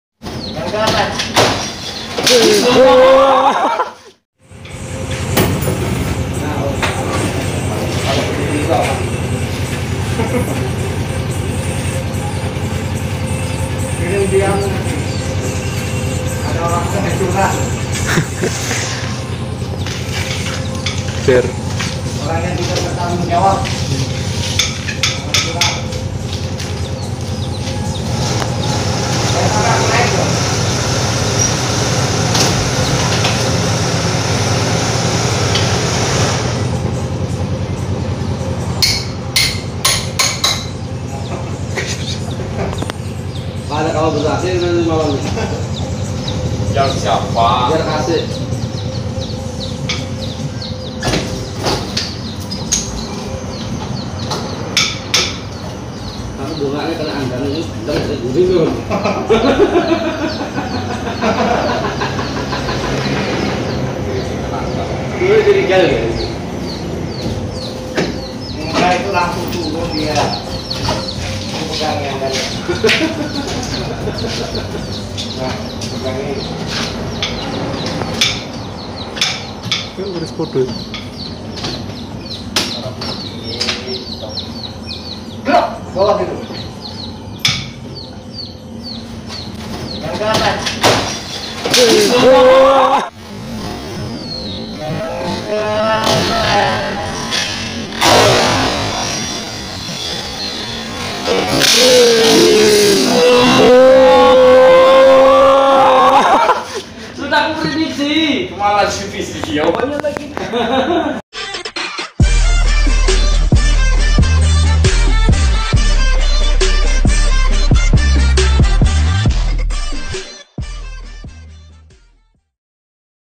Menegangkan detik-detik potong beton
Menegangkan detik-detik potong beton rumah tp udah dipecah betonnya